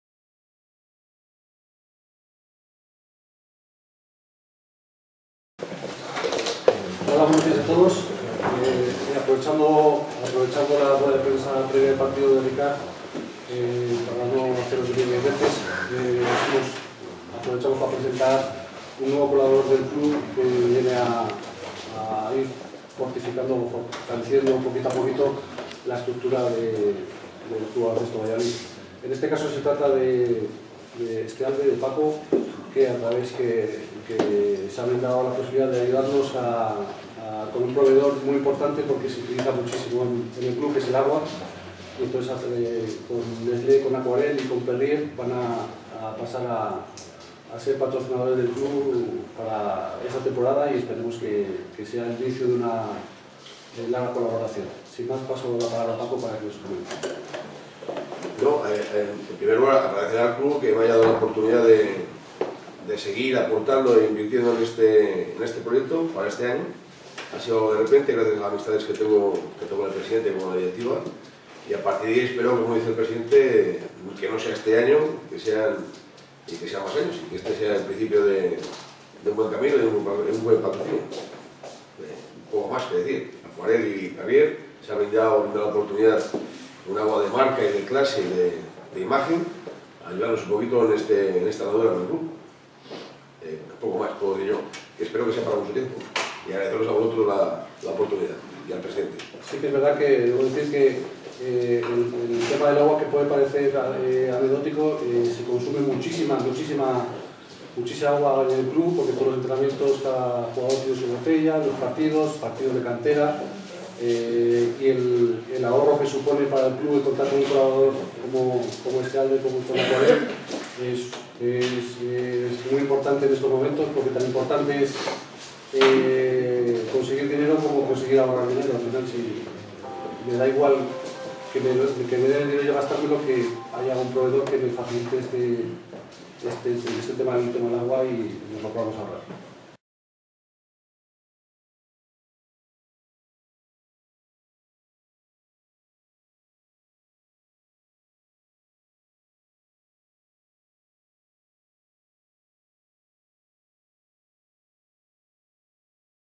Presentación de Extealde
El presidente de la Fundación Baloncesto Valladolid, Juan Vela, ha presentado esta mañana a nuestro nuevo distribuidor oficial de agua, Extealde.